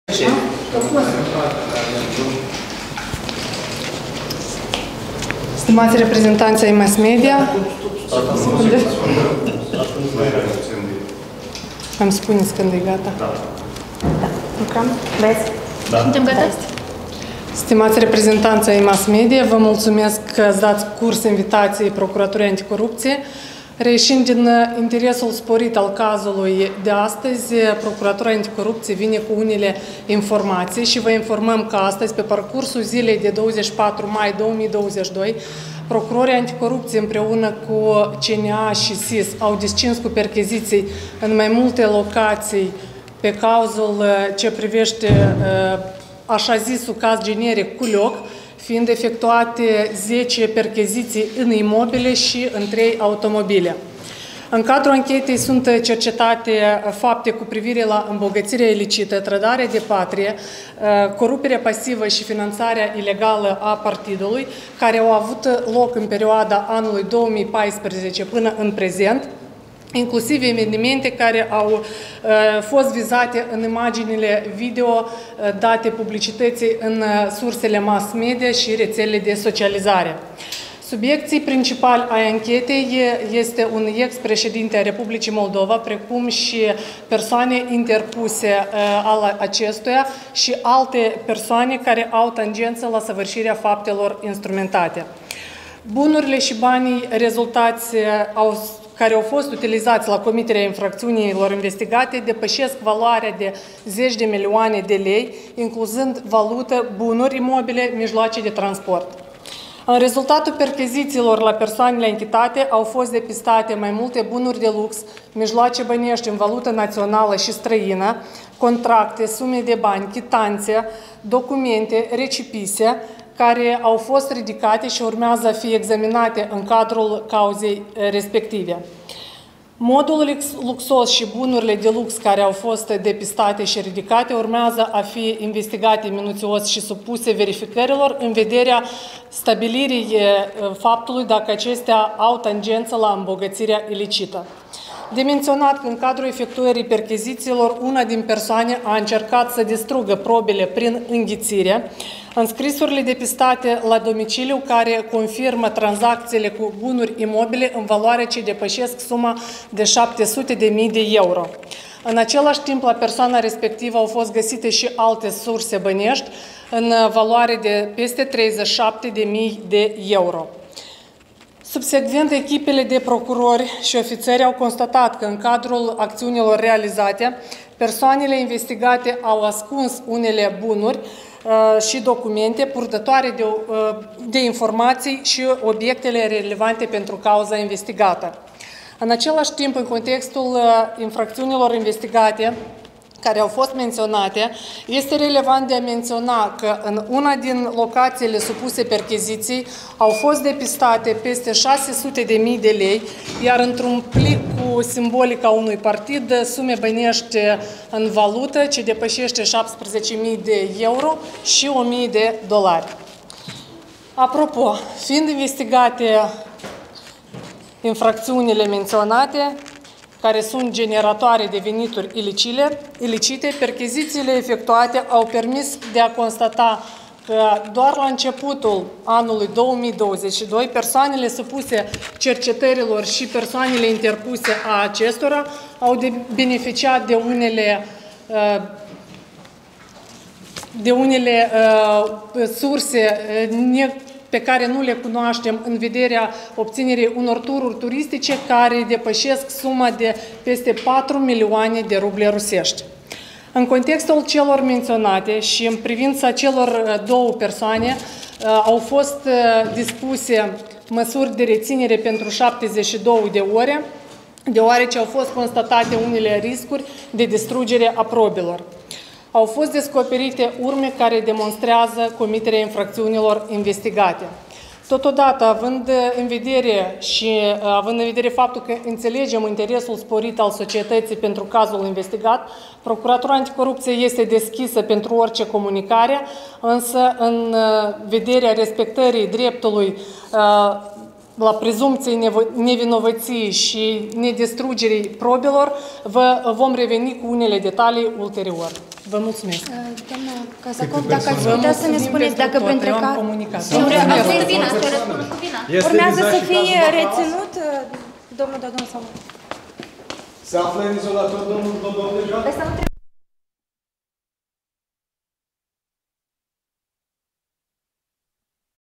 Briefing susținut de Procuratura Anticorupție privind perchezițiile la ex-președintele Igor Dodon
Briefing de presă susținut de Procuratura Anticorupție cu tema „Rezultatul perchezițiilor efectuate la ex-președintele Republicii Moldova, Igor Dodon”